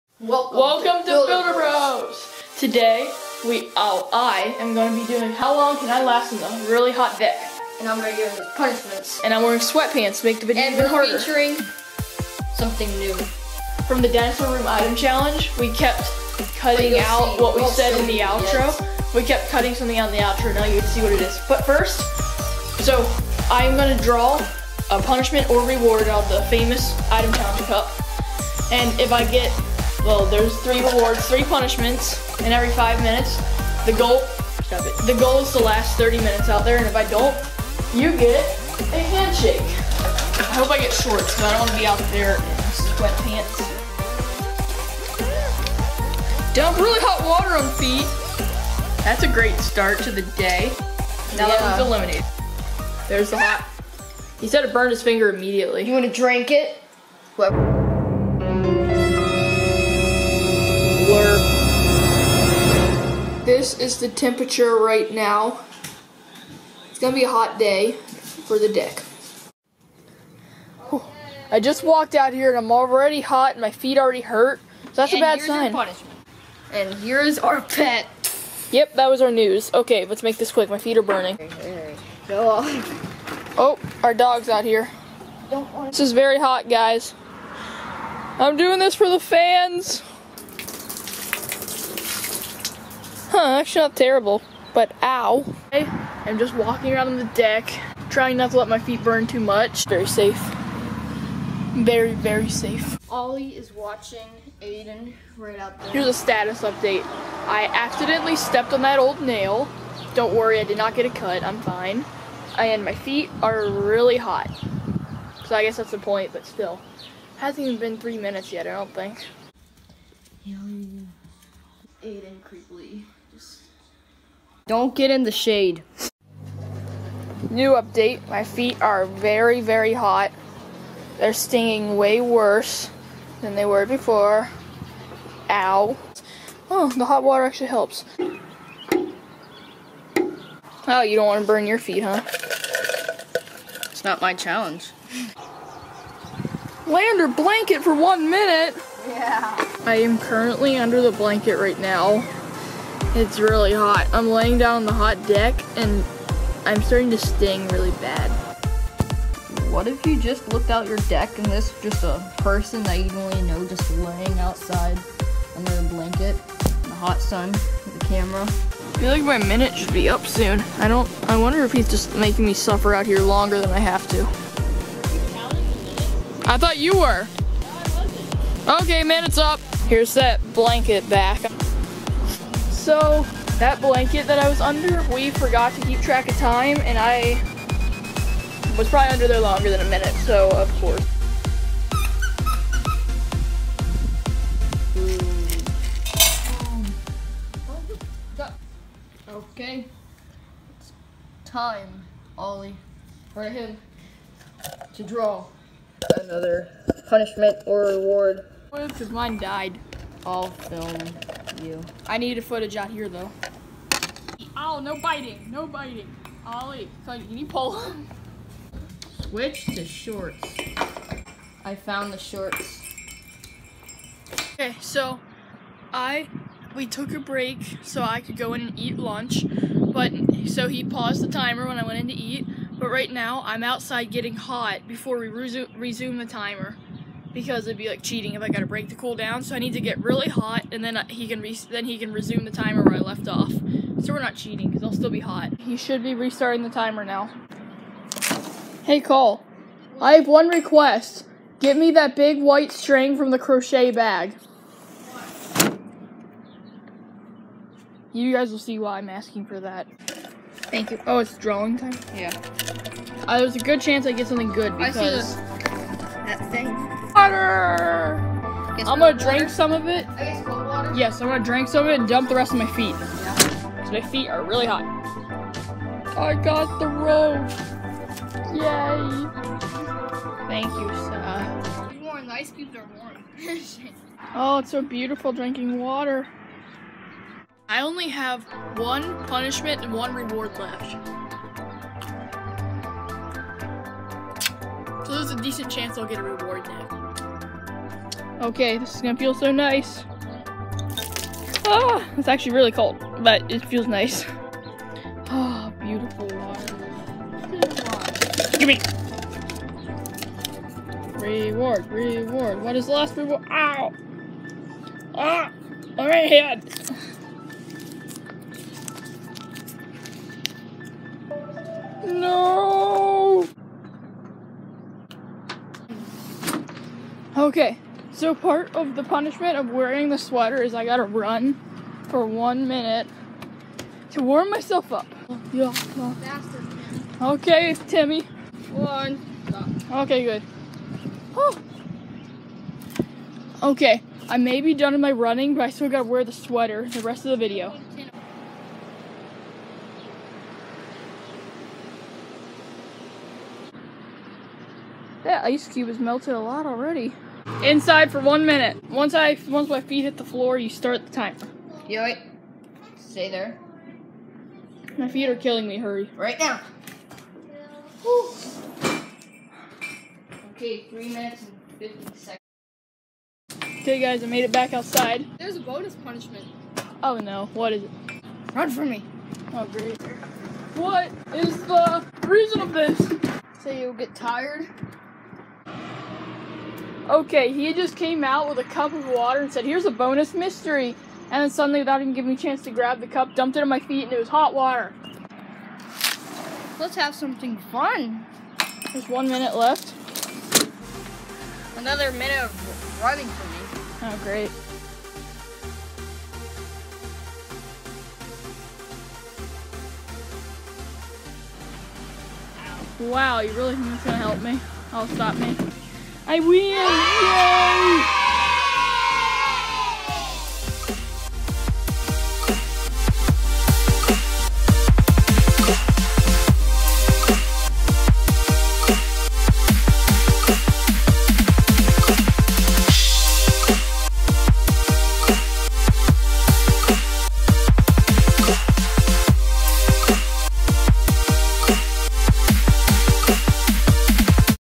Can I Survive The HOT Deck!?
I am on a HOT deck in this video. My goal is to last 30 minutes, but there’s a catch, I have punishments to make it harder! There are also some rewards to make it easier.